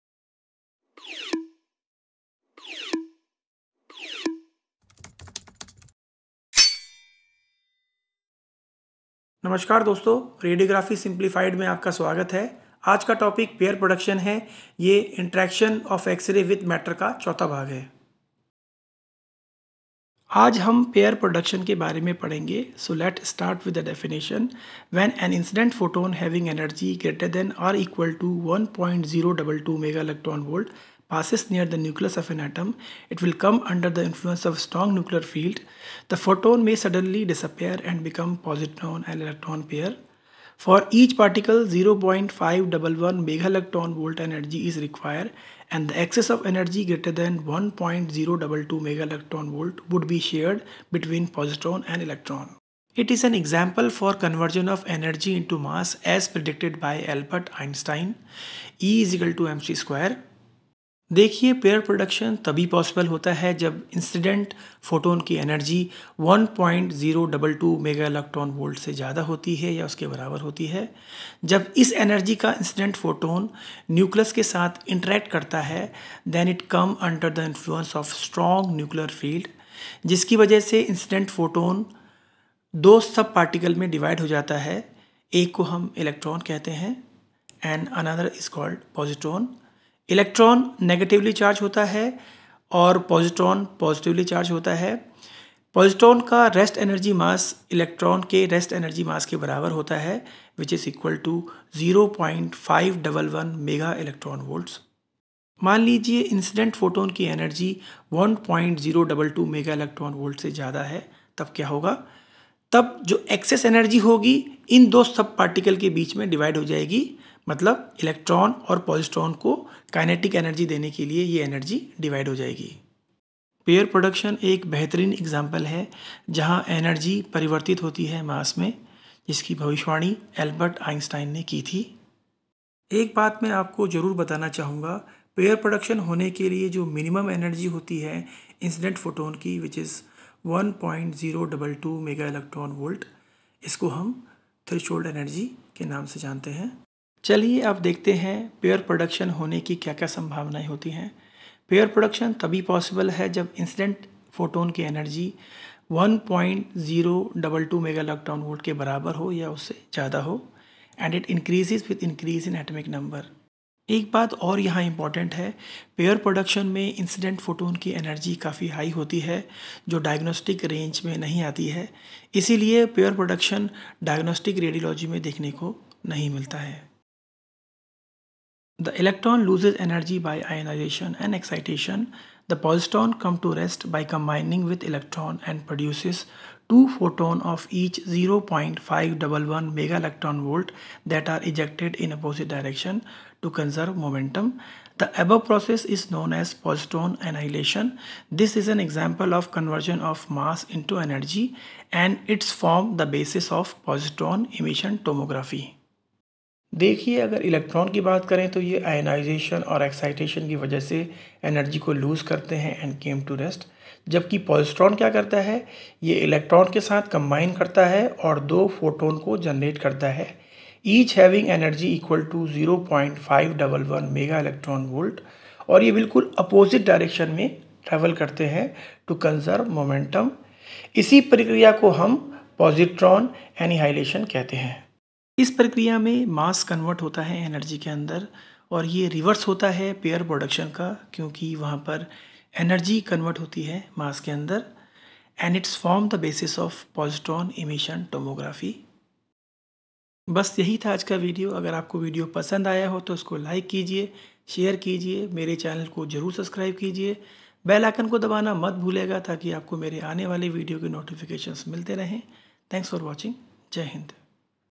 Pair production & positron annihilation l interaction of x-ray with matter l Radiography Simplified
Hi friends!

Old video links

Today I am posting video on pair production and positron annihilation.

When a photon having energy greater than or equal to  1.022 MeV, passes near the nucleus of an atom, will come under the influence of strong nuclear field.. The photon may suddenly disappear and become a positron and electron pair. For each particle 0.511 MeV energy is required and the excess energy
Greater than 1.022 MeV, would be shared between the positron and electron as kinetic energy.

 Positron annihilation-:  here matter is converted into photon.

Take care
Bye.